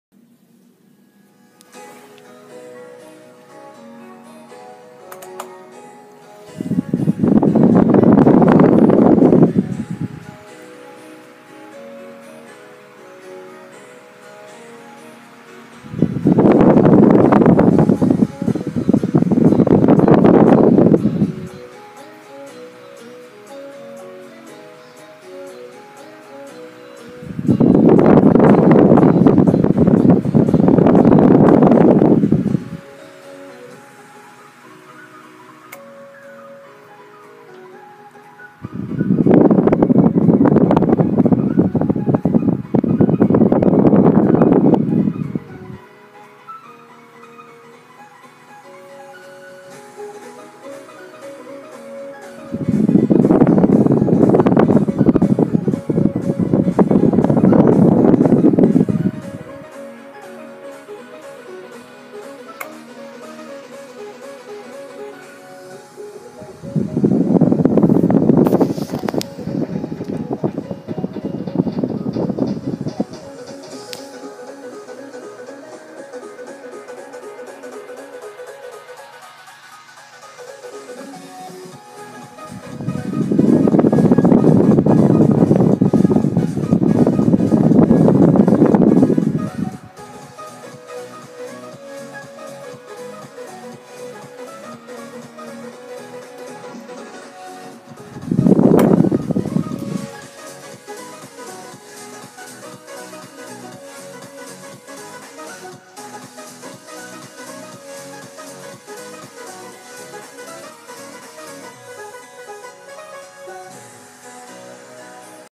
(NEW FAN) 12 White Westinghouse Desk Fan
Model: W12DR7
Size: 30cm 305 mm (12")
Power Consumption: 120 Watts
Year: 2017
Color: White and Badge
Previous day we clean this fan